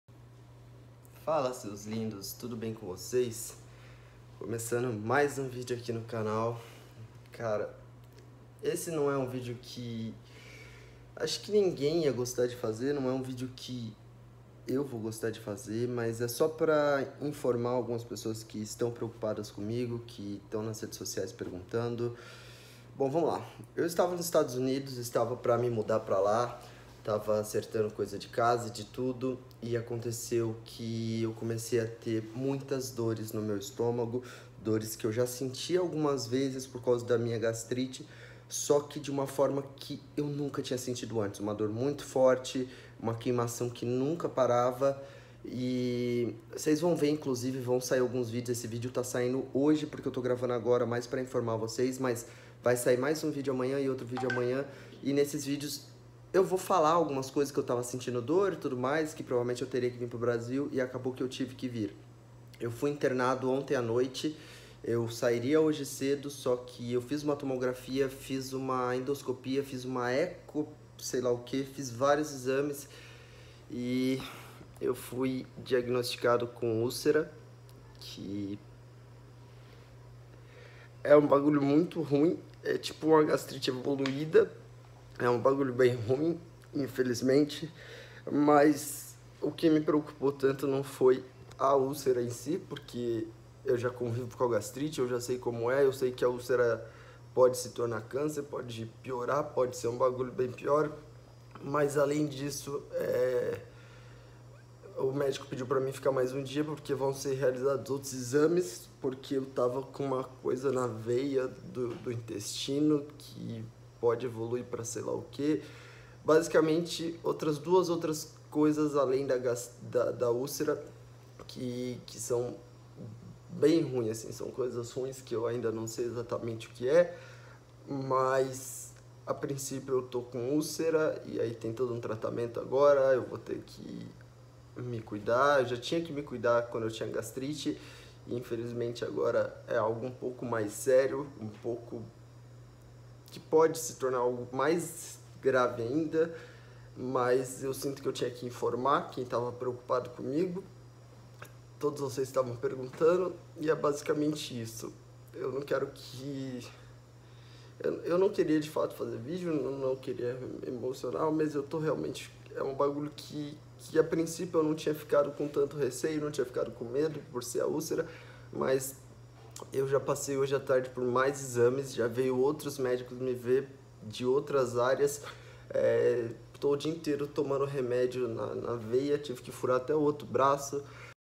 fui internado :(
Apenas uma atualização rápida, infelizmente tive que voltar dos estados unidos e descobriram algo muito ruim, mas tenho certeza que vou melhorar. Obrigado por se preocuparem e os próximos videos vão sair normal, e vai dar pra entender um pouco do que aconteceu.